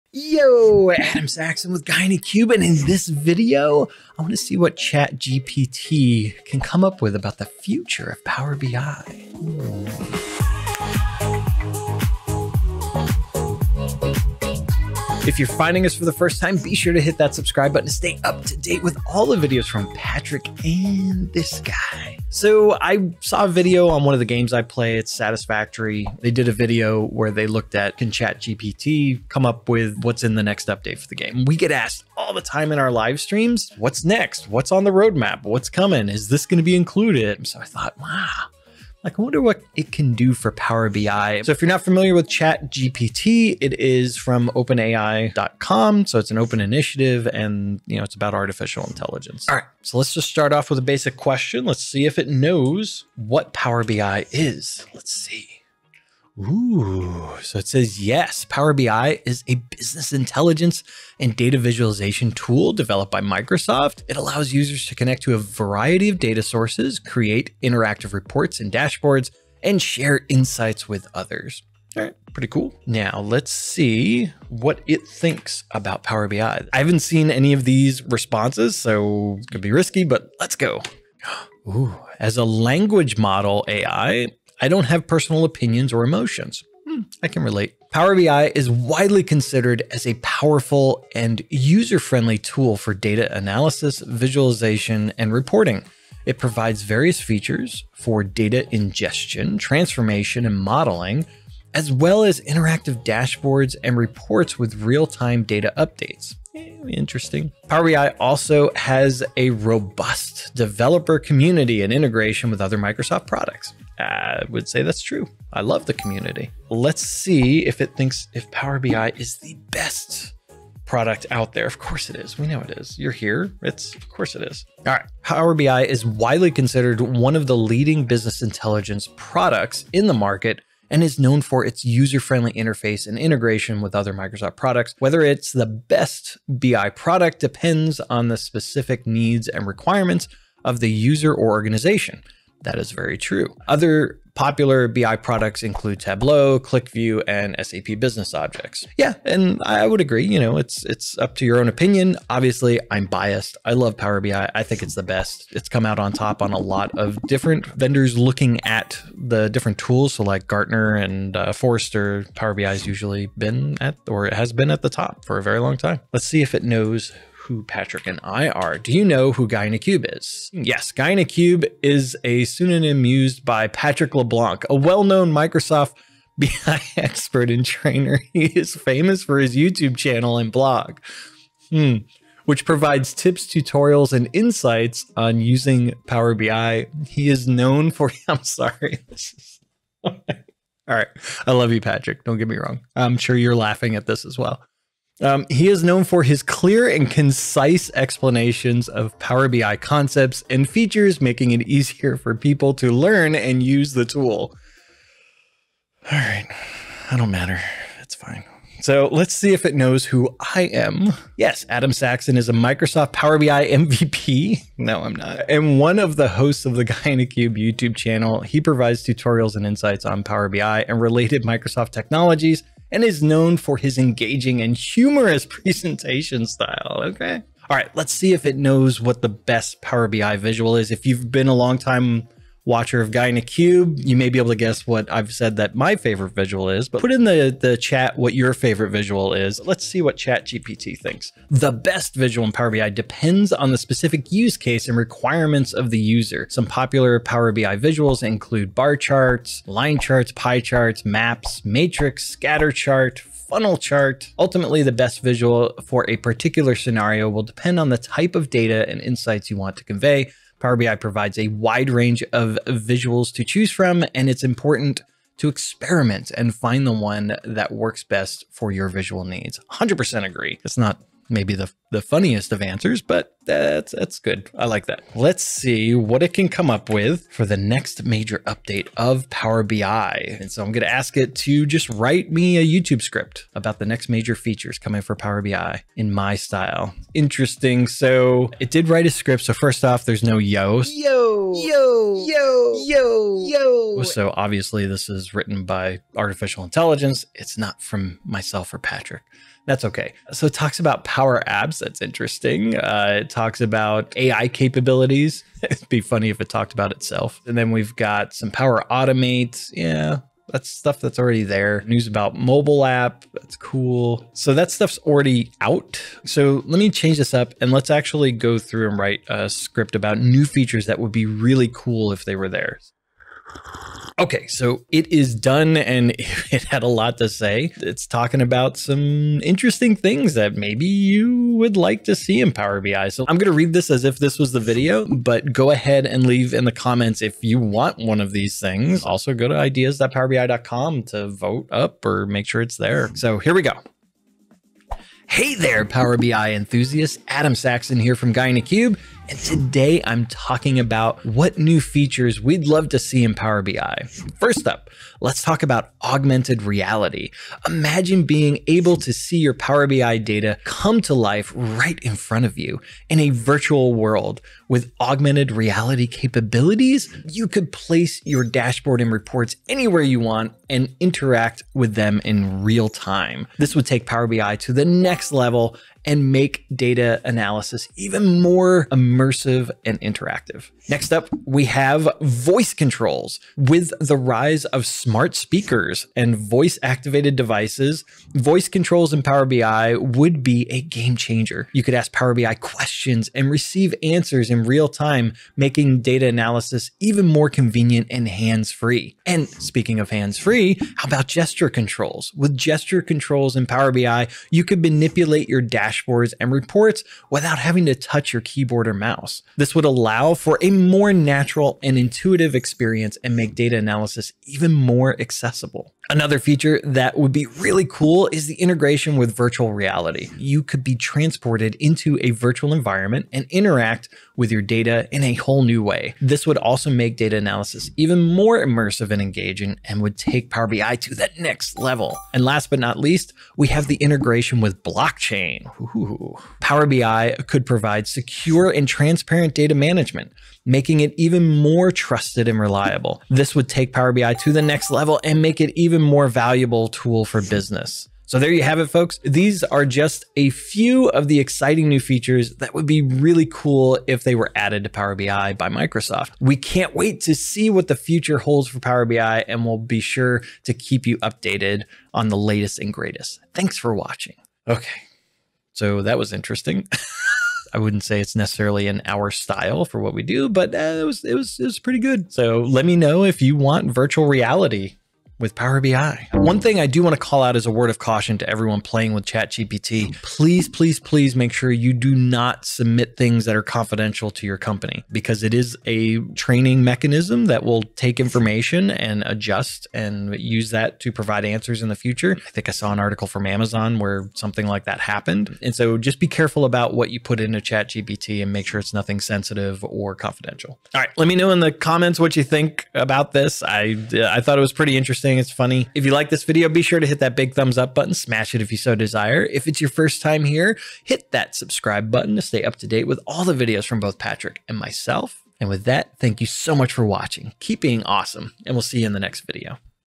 Let's see if ChatGPT knows what's coming next for Power BI...
Everyone is making videos on ChatGPT and trying to solve problems. What about telling the future? Let's see if ChatGPT can tell us what's coming next for Power BI!